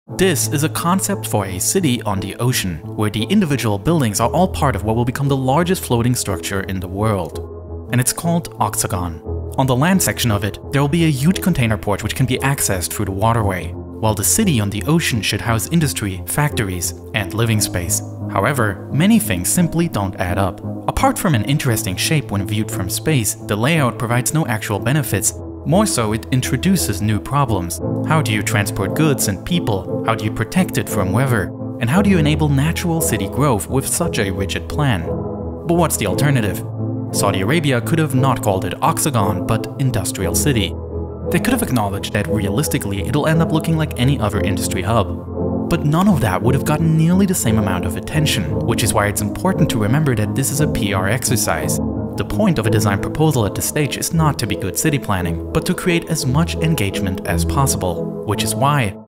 Saudi Arabia Wants to Build a City in the Ocean
Saudi Arabia announced that the kingdom wants to build an octagonal city called Oxagon which, amongst the Line and Trojena, will be part of the NEOM project spearheaded by crown prince Mohammed bin Salman. But how good of an idea is it in terms of city planning to build in the ocean?

I have a ton of full lenght exclusive videos that are not on YouTube, go check them out on Nebula: